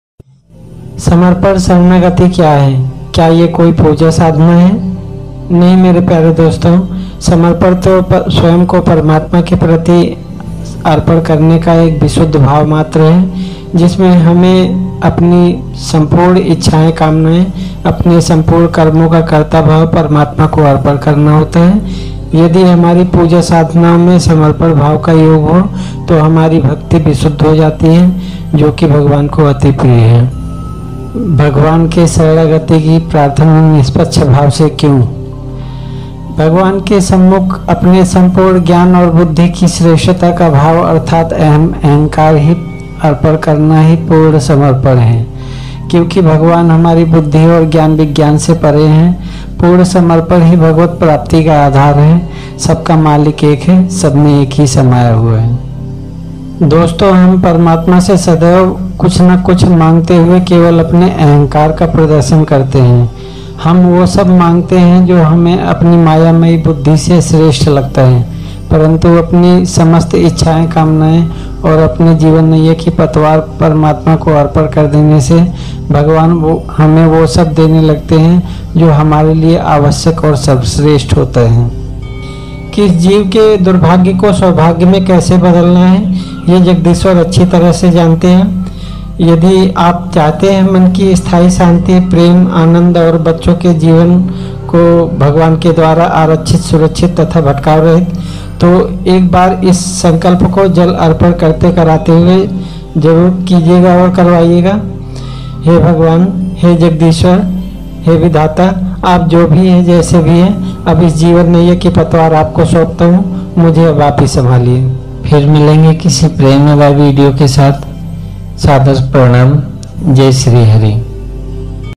समर्पण शरणागति क्या है | समर्पण निष्पक्ष भाव से क्यों | स्थाई शान्ति कैसे मिले | Prabhu sharanagat
important message -:

जय जय श्री राधे कृष्णा मेरे प्यारे भाइयों बहनों

अपने प्रियजनों और बच्चों का जीवन भी  भगवान के द्वारा आरक्षित सुरक्षित तथा भटकाव रहित बनाने के लिए

 महीने में एक बार ही सही इस संकल्प को जल अर्पण करते कराते हुए जरूर कीजिए करवाइएग।

"हे भगवान" "हे जगदीश्वर"  अब इस जीवन नैया की पतवार आपको सौंपता हूं  मुझे अब आप ही संभालिए ।

 क्योंकि हमारी सन्तान  भी वास्तव में एक जीवात्मा ही हैं।
 जो कलयुगी प्रभाव से अपने ही कर्मानुसार भले-बुरे प्रारब्ध और अवगुणों को भी लेकर जन्मे हैं।

अतः कब कौन सी अनहोनी और दुर्गुण उनके जीवन को प्रभावित करे उससे पूर्व ही उन्हें भगवान के द्वारा आरछित सुरक्षित करा दें।

क्योंकि एक भगवान के अतिरिक्त कोई और हमारे दुर्भाग्य को टाल नहीं सकता।

जब तक हम जीवात्माएं अपने अहं अहंकार से जुड़े होते हैं तब तक "श्री भगवान" कुछ नहीं करते हैं।

परंतु जब हम पूर्ण श्रद्धा और विश्वास से अपने जीवन नैया की पतवार प्रभु जी को सौंप देते हैं तो वहीं से प्रभु जी हमारा हाथ थाम लेते हैं

फिर वही होता है जो श्री भगवान चाहते हैं।

जो कि हमारे लिए सर्वश्रेष्ठ होता है

Special Thanks to the original creator/author of the video clip and picture and background music used in my video.